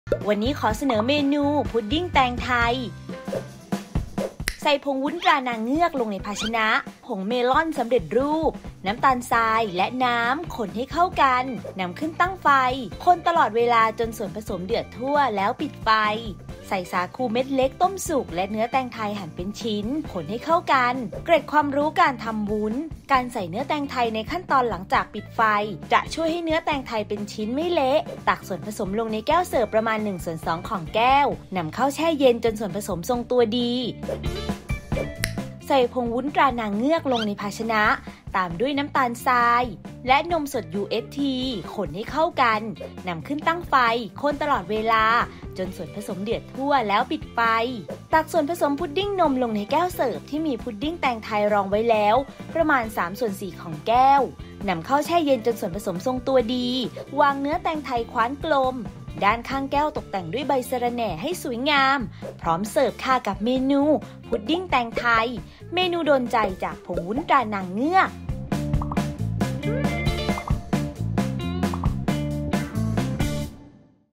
พุดดิ้งแตงไทย ทำง่ายๆ สไตล์ อ.ยิ่งศักดิ์
พุดดิ้งแตงไทย (ผลิตภัณฑ์: ผงวุ้น ตรานางเงือก)

ผงเมล่อนสำเร็จรูป  1 ช้อนโต๊ะ
สาคูเม็ดเล็กต้มสุก  150 กรัม
เนื้อแตงไทยหั่นเป็นชิ้นสี่เหลี่ยมเล็ก 50 กรัม
แก้ว สำหรับเสิร์ฟ
สิ่งที่ต้องเตรียม (หน้าพุดดิ้งนม)
นมสดยูเอชที   350 มิลลิลิตร
เนื้อแตงไทยคว้านกลมและใบสะระแหน่ สำหรับตกแต่ง

1.ใส่ผงวุ้น ตรานางเงือกลงในภาชนะ ตามด้วยผงเมล่อนสำเร็จรูป น้ำตาลทรายและน้ำ คนให้เข้ากัน นำขึ้นตั้งไฟ คนตลอดเวลาจนส่วนผสมเดือดทั่ว ปิดไฟ
2.ใส่สาคูเม็ดเล็กต้มสุกและเนื้อแตงไทยหั่นเป็นชิ้น คนให้เข้ากัน (การใส่เนื้อแตงไทยในขั้นตอนหลังจากปิดไฟจะช่วยให้เนื้อแตงไทยเป็นชิ้นไม่เละ)ตักส่วนผสมลงในแก้วเสิร์ฟประมาณ ½ ของแก้ว นำเข้าแช่เย็นจนส่วนผสมทรงตัวดี เตรียมไว้

วิธีทำ(หน้าพุดดิ้งนม)
1.ใส่ผงวุ้น ตรานางเงือกลงในภาชนะ ตามด้วยน้ำตาลทรายและนมสดยูเอชที คนให้เข้ากัน นำขึ้นตั้งไฟ คนตลอดเวลาจนส่วนผสมเดือดทั่ว ปิดไฟ
2.ตักส่วนผสมพุดดิ้งนมลงในแก้วเสิร์ฟที่มีพุดดิ้งแตงไทยรองไว้แล้ว ประมาณ ¾ ของแก้ว
3.นำเข้าแช่เย็นจนส่วนผสมทรงตัวดี วางเนื้อแตงไทยคว้านกลม ด้านข้างแก้วตกแต่งด้วยใบสะระแหน่ให้สวยงามพร้อมเสิร์ฟ

▲Line @yingsakfood